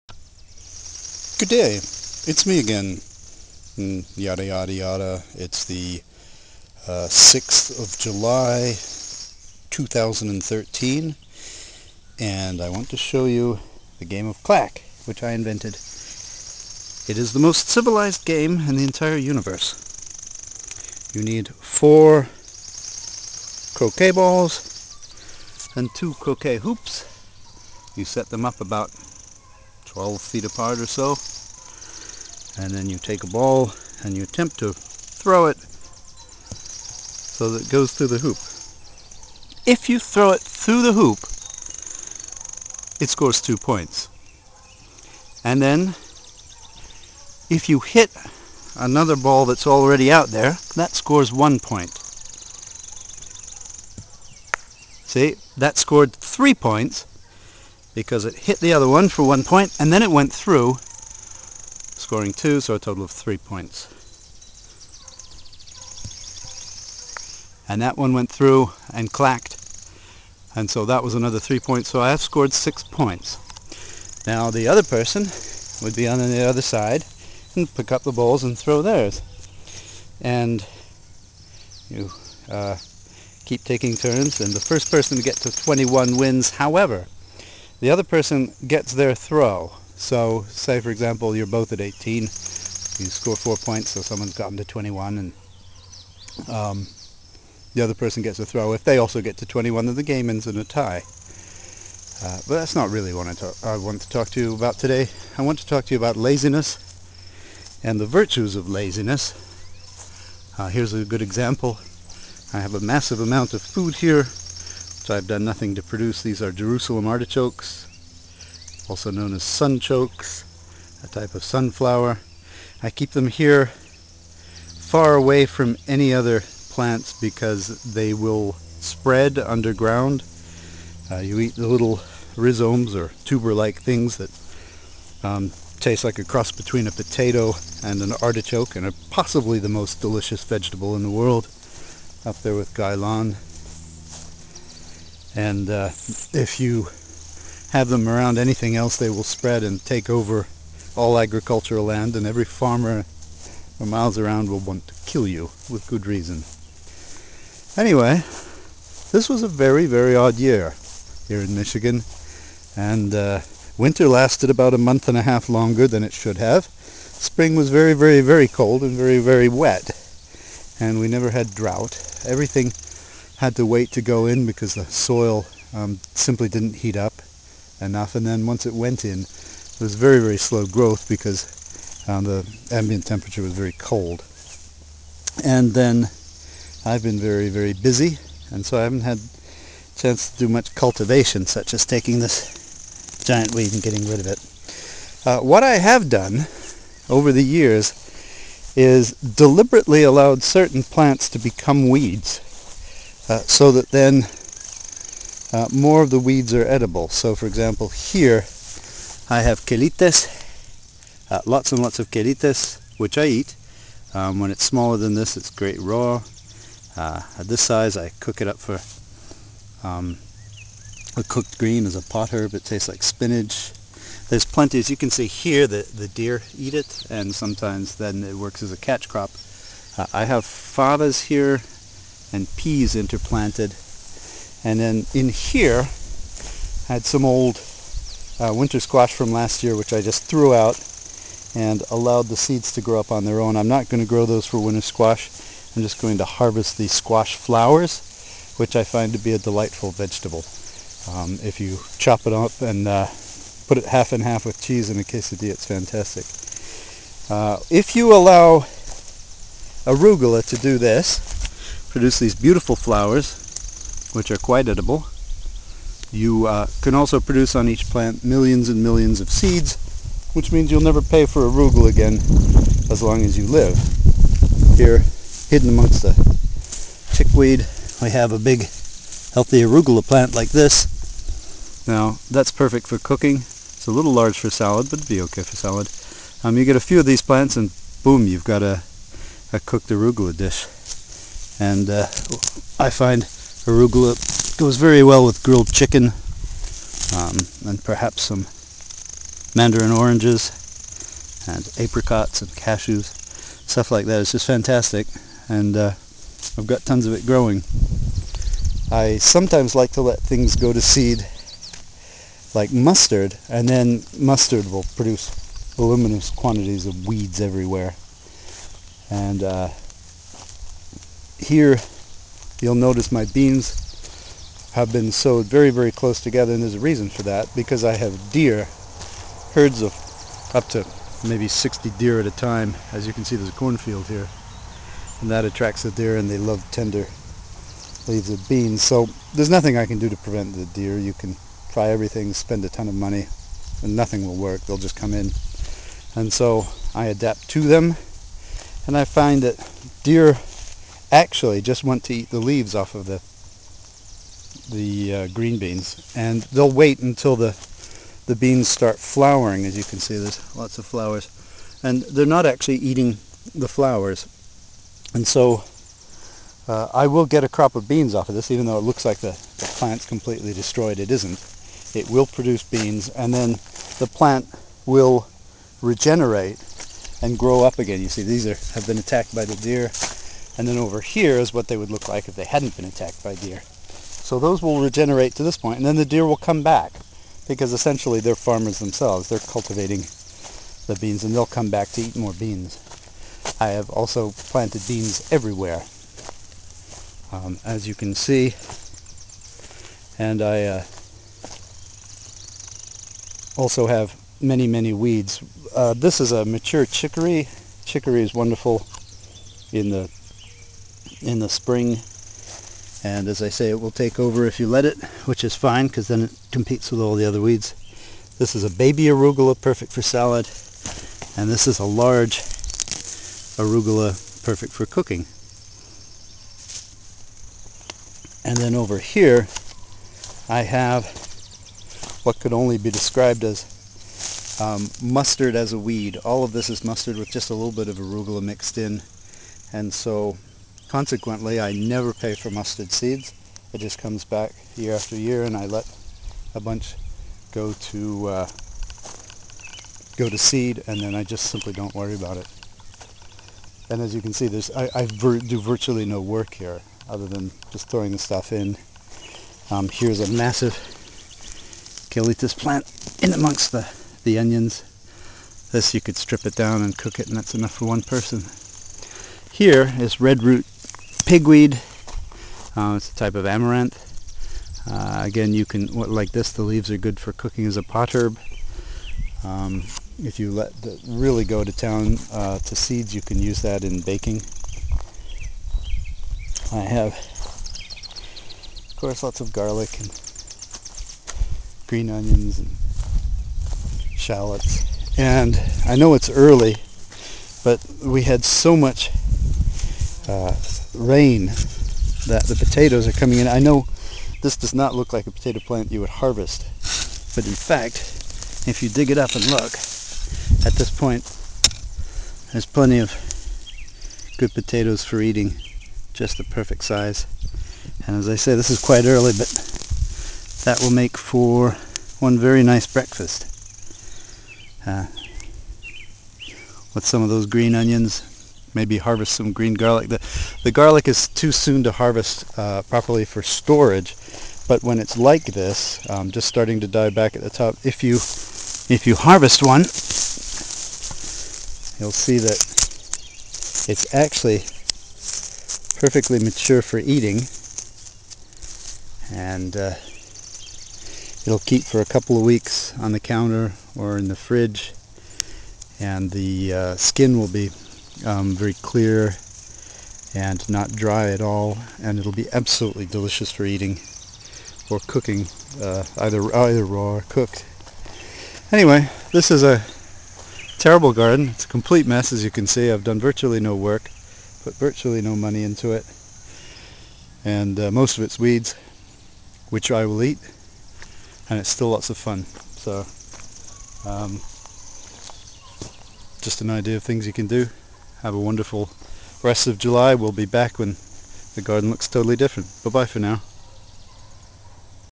Weeds: an apology (with clack)
An ode to laziness in the summer, and gluttony and idleness, too. My whole point is to have weeds which are edible and which drive out inedible weeds, so that as much as possible cultivation IS harvesting. Two short inventories:

Weeds I allow to flourish so I can eat them:  purslane (verdolagas), lamb's quarters (quelites), redroot pigweed (amaranth), sorrel, dandelion.

Foods I allow to become weeds so I can eat them without doing any work: arugula, mustard, cilantro, rapini, cress, cherry tomatoes, dill.  And under very controlled circumstances: mint, jerusalem artichokes.